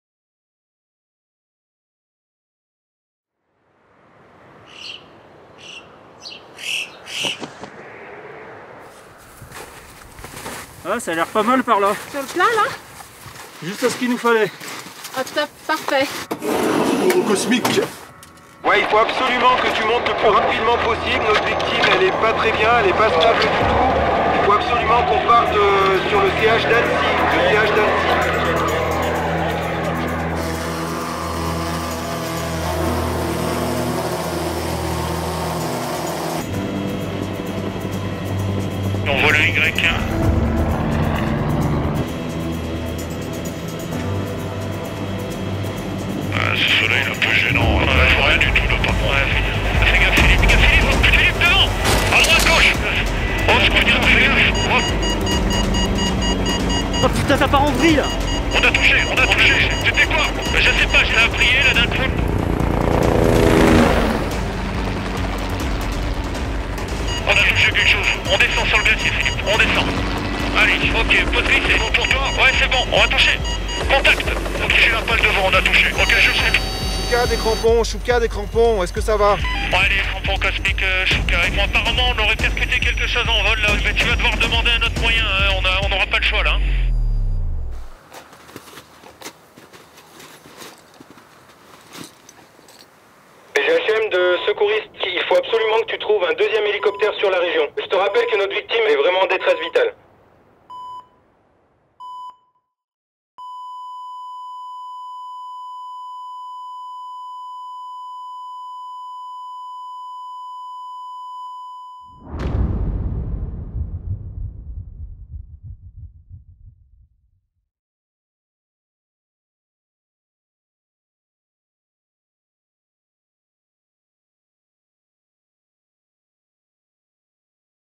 🇬🇧 Drone vs. helicopter: an unsuspected danger
🇬🇧 Helicopters fly at all altitudes in the mountains,
Drone are invisible for the pilot and crew,
A collision can jeopardize their mission...